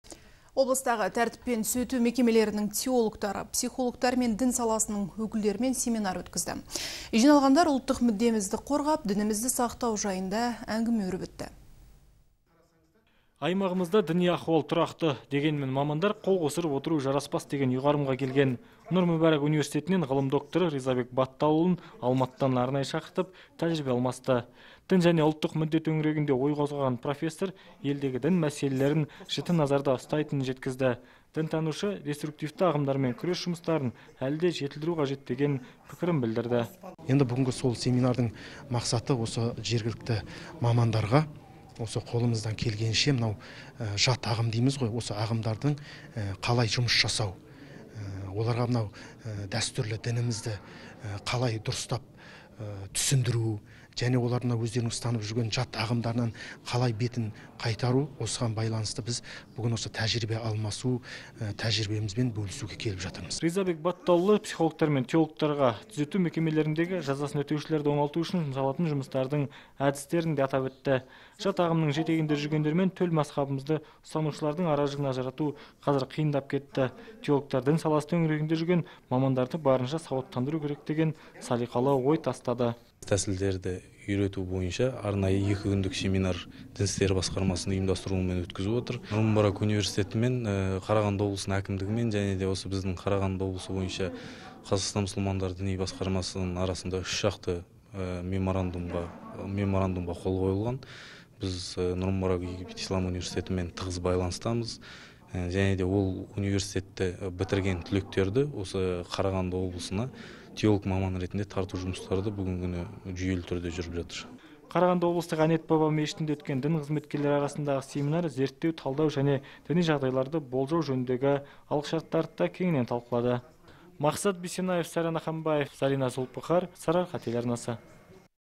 Облыстық орталық мешітінде семинар-практикум өтті
2019 жылдың 26-27 сәуір күндері Қарағанды облысының Дін істері басқармасының ұйымдастыруымен «Әнет баба» атындағы облыстық орталық мешітінде семинар-практикум өткізілді. Семинарға Қарағанды облысындағы қала мен аудандарда, түзету мекемелерінде жұмыс атқаратын теолог, дінтанушы мамандар қатысып, оның негізгі лекторы ислам ғылымдарының докторы, «Нұр Мүбарак» университетінің ұстазы Ризабек Батталұлы болды.
Семинар барысында радикалды идеология ұстанушы азаматтармен қазіргі кезде жасалып жатқан жұмыстардың тиімділігін арттыру бойынша жаңа әдістемелер талқыланып, қатысушылар арасында тренинг өткізілді. Сонымен қатар, Р.Балталұлы түзету мекемелерінде жұмыс ерекшелігі туралы айта отыра, радикалды ағымдардың жақтаушылары жиі қолданатын діни терминдер туралы айтып, оларға жан-жақты түсініктеме берді.